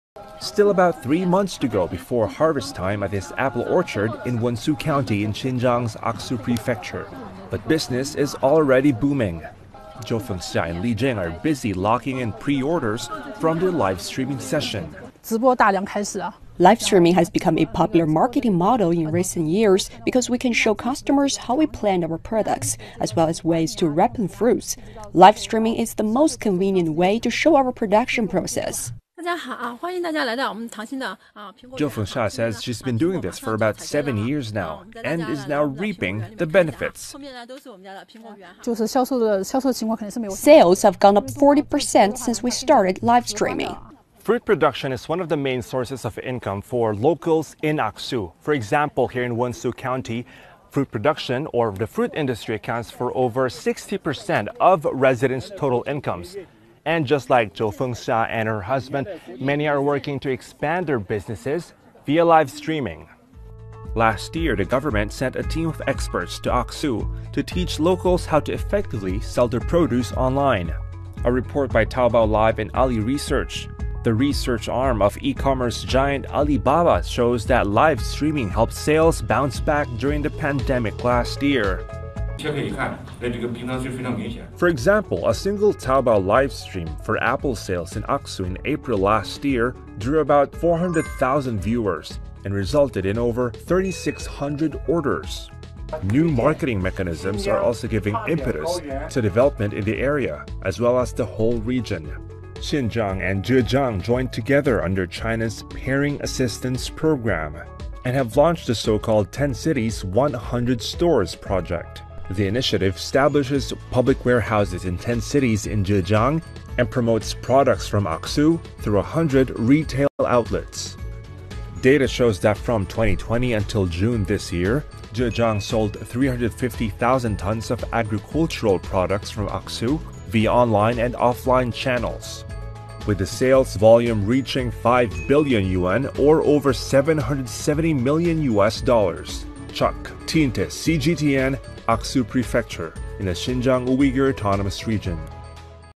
Livestreaming, new sales mechanisms boost fruit industry in Aksu
Livestreaming and new marketing and sales strategies are boosting fruit industries in Xinjiang. We visit an apple orchard in the region's Aksu Prefecture.